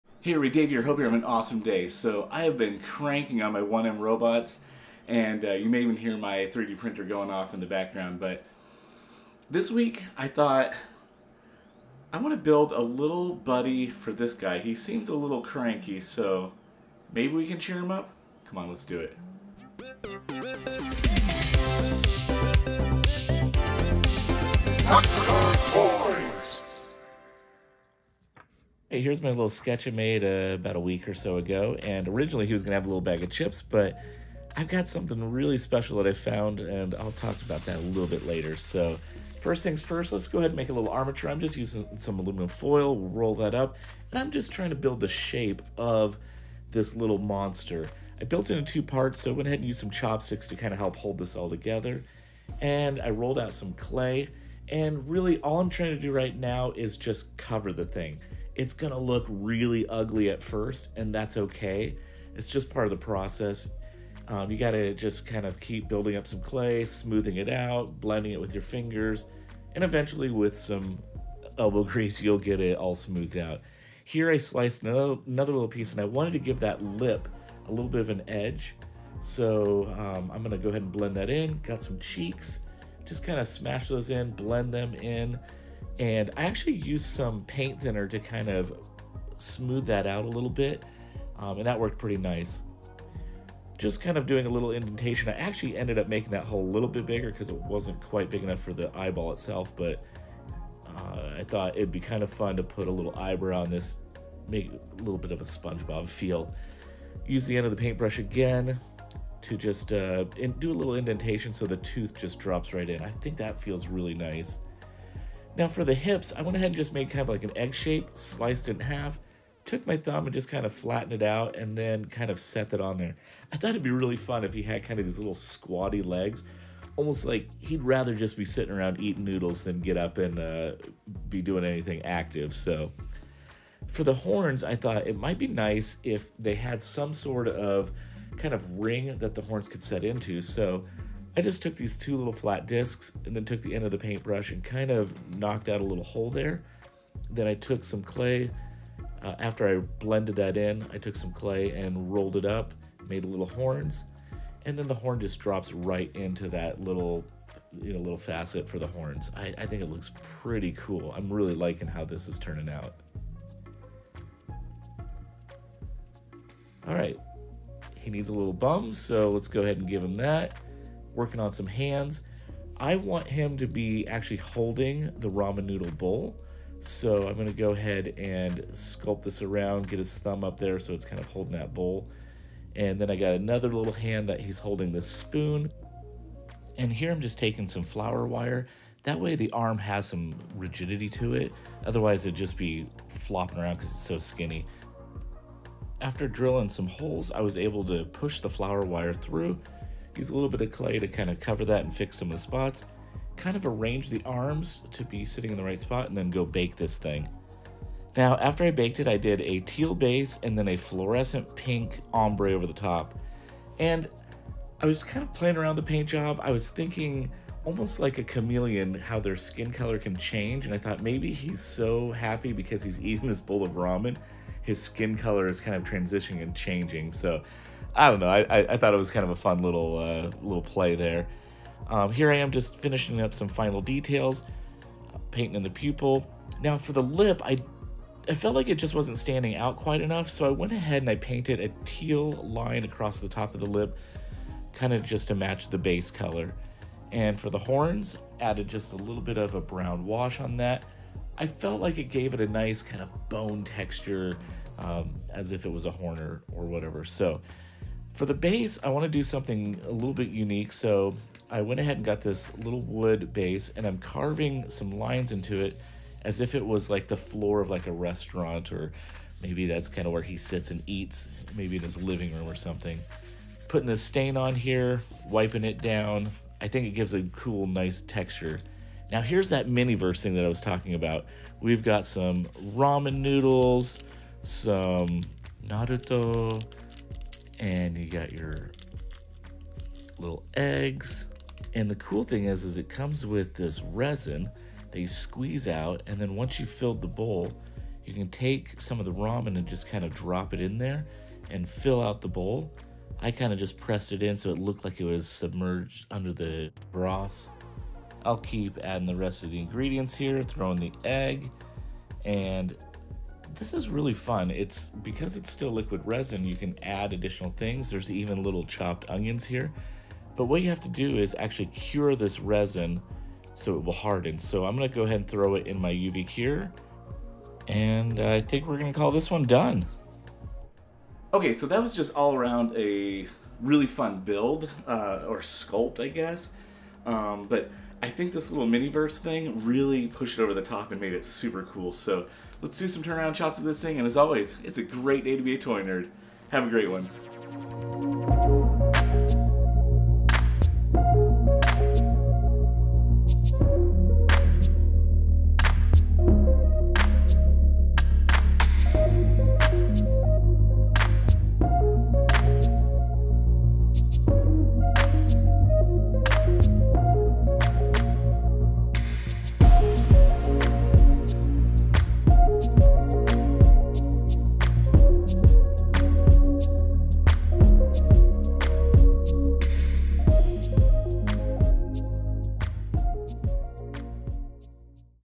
Sculpting a Ramen-Loving Monster Friend for My Little Devil
My Little Devil seemed a little lonely, so I decided to sculpt him a little buddy. I sketched up a monster that loves to eat, and I had a ton of fun sculpting him enjoying a huge bowl of ramen. I used aluminum foil for the armature and Super Sculpey for the body. I also used a Mini Verse resin toy kit that my daughter found at Target to add some fun details.

Materials:

Super Sculpey
Sculpting tools
Paint (optional)
Instructions:

Start by sketching out your design. This will help you to visualize your sculpture and make sure that all of the proportions are correct.
Once you have a sketch, begin to sculpt your devil monster out of Super Sculpey. You can use your fingers or sculpting tools to create the desired shapes and textures.
Once you are happy with your sculpture, bake it in the oven according to the instructions on the Super Sculpey packaging.
Once your sculpture is baked, you can paint it (optional).
This is a fun and easy project that is perfect for people of all skill levels. Get creative and have fun!

I hope you enjoy this custom sculpting video!
Thanks for visiting DR Toys, and as always...

"It's a great day to be a toy nerd!"